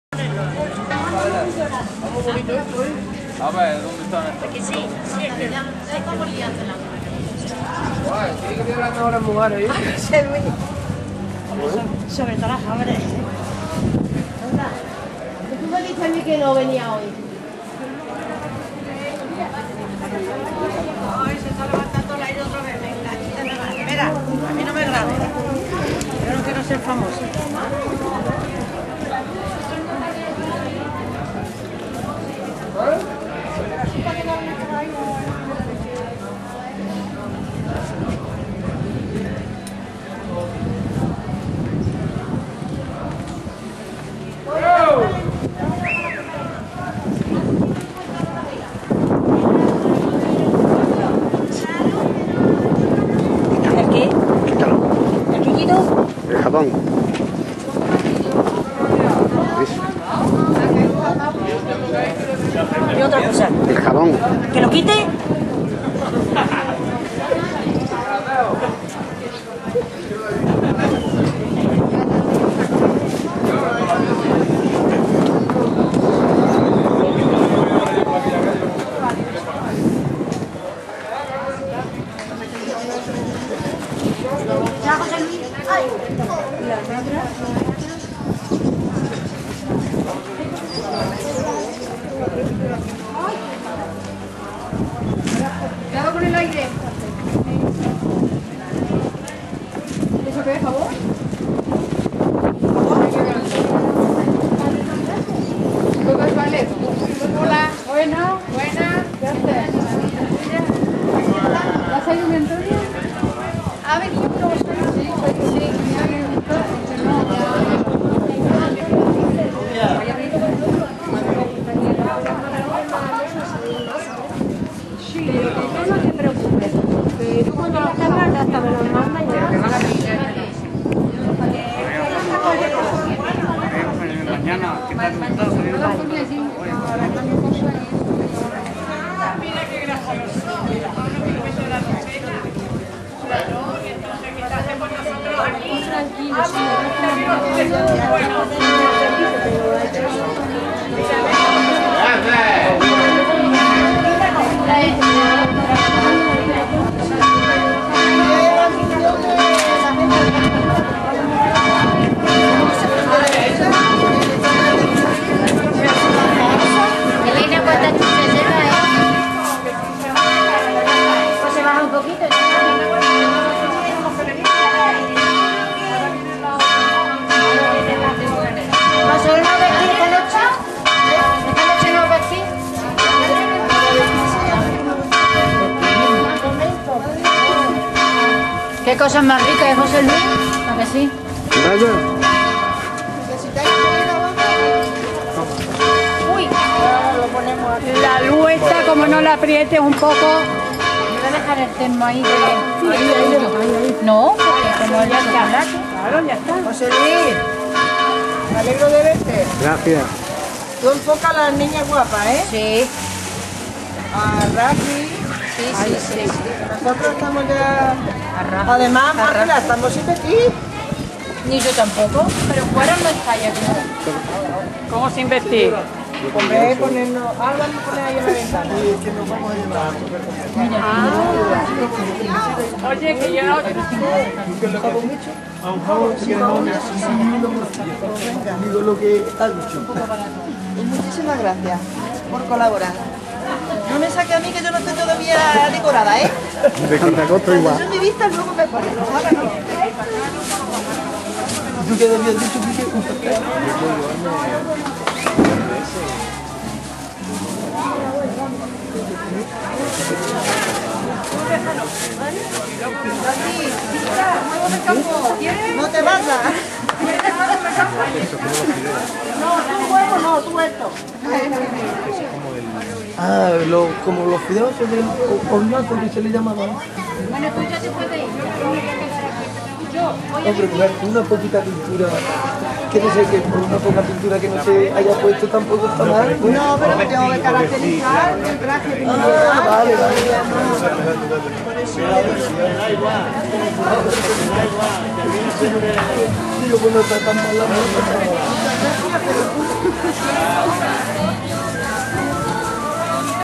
Mercado Franco...Candelaria 2013(II). Dos Torres(Córdoba)
Dia de la Candelaria 1 y 2 de Febrero, con muestra Gastronomica, concierto de "Jara y Granito", y el Grupo "Nostalgia". Con Mercado Franco, gran Fogata, juegos tradicionales...Visita teatrilizada.